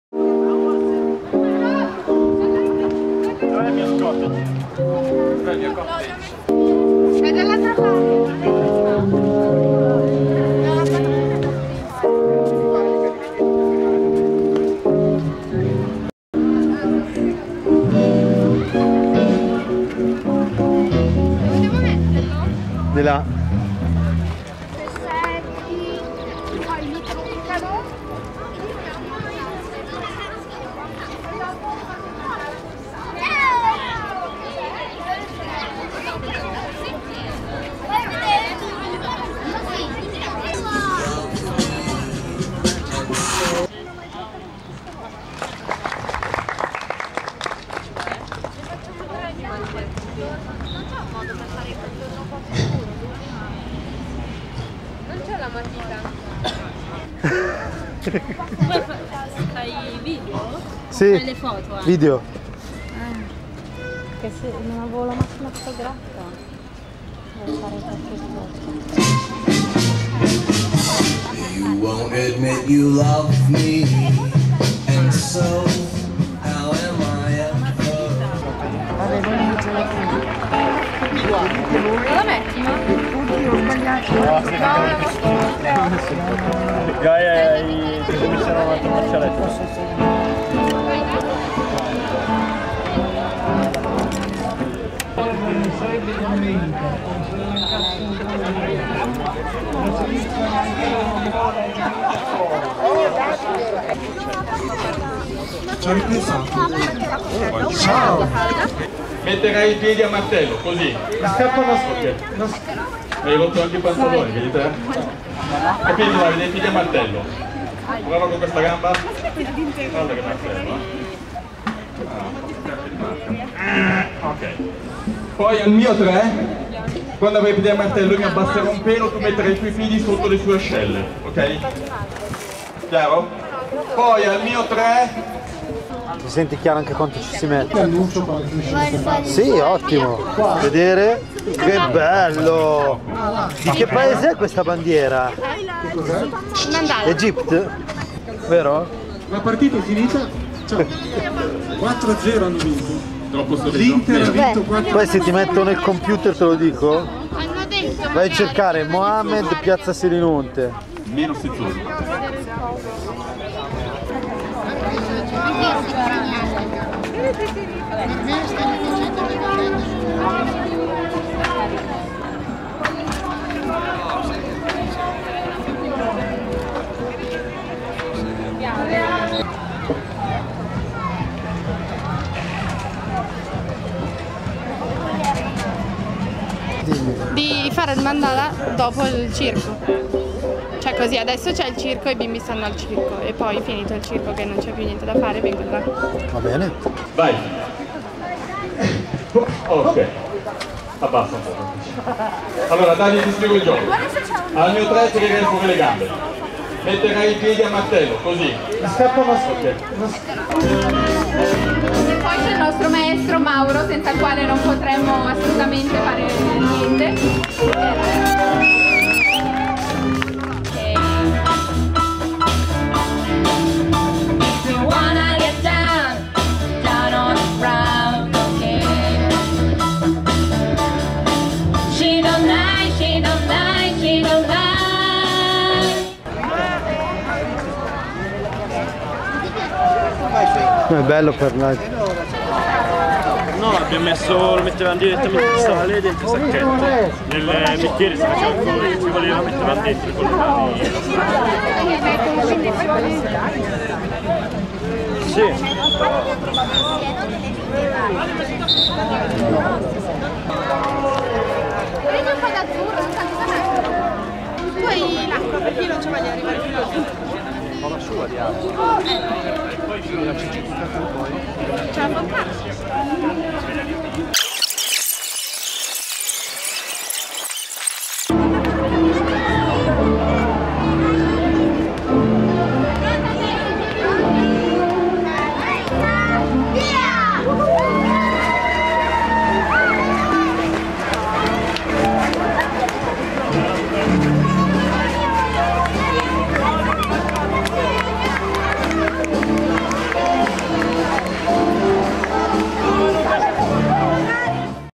R&M @ Pazza Piazza 14 maggio 2012
Progetto Riti & Miti della Cultura Giovanile
di Matteo Saltalamacchia Vincenzo Monte e Francesco Blasa al Liceo Vittorio Veneto

Azione di Coesione Sociale tra i ragazzi del Liceo e la popolazione del Quartiere Popolare vicino, 'Quartiere San Siro', nella piazza-epicentro Selinunte.

All'interno dell'iniziativa Rara Razza di Pazza Piazza promossa dal Cps di Zona nell'ambito delle attività per la sensibilizzazione e la prevenzione ai problemi di Salute Mentale.